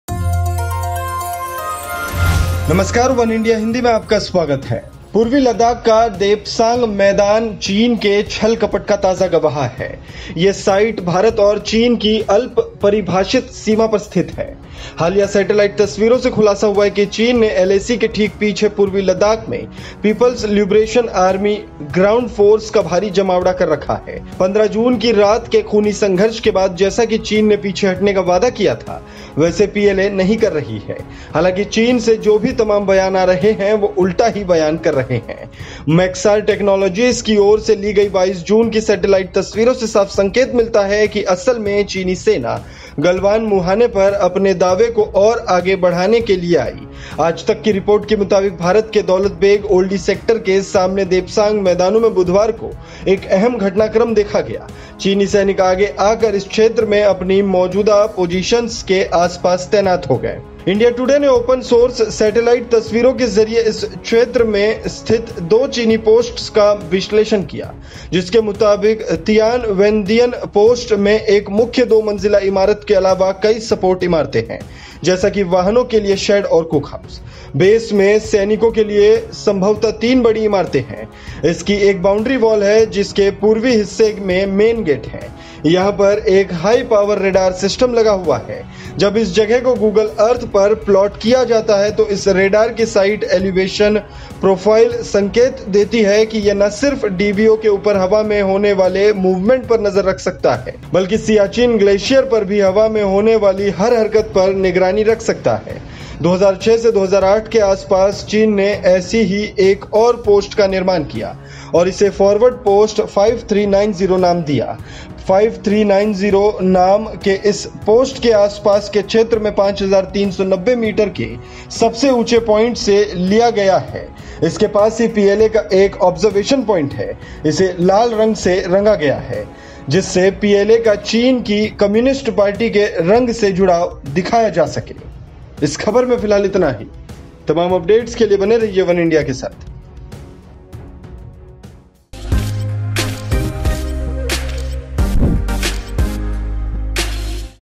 India China Tension: सैटेलाइट तस्वीरों में देखें पूर्वी लद्दाख में PLA का जमावड़ा | वनइंडिया हिंदी
The Depsang Plains in eastern Ladakh is the latest site of Chinese treachery on the ill-defined border with India. Recent satellite images have revealed that China has amassed the People's Liberation Army Ground Force (PLAGF) troops in eastern Ladakh right behind the Line of Actual Control (LAC). After the bloodyfight on the fateful night of June 15, 2020, it came to light that the PLA is not moving back as promised, although all the statements coming out of China are claiming the opposite.

देपसांग मैदान चीन के छल-कपट का ताजा गवाह है. ये साइट भारत और चीन की अल्प-परिभाषित सीमा पर स्थित है. हालिया सैटेलाइट तस्वीरों से खुलासा हुआ है कि चीन ने LAC के ठीक पीछे पूर्वी लद्दाख में पीपुल्स लिबरेशन आर्मी ग्राउंड फोर्स का भारी जमावड़ा कर रखा है. 15 जून की रात के खूनी संघर्ष के बाद, जैसा कि चीन ने पीछे हटने का वादा किया था, वैसे PLA नहीं कर रही है. हालांकि चीन से जो भी तमाम बयान आ रहे हैं वो उलटा ही बयान कर रहे हैं.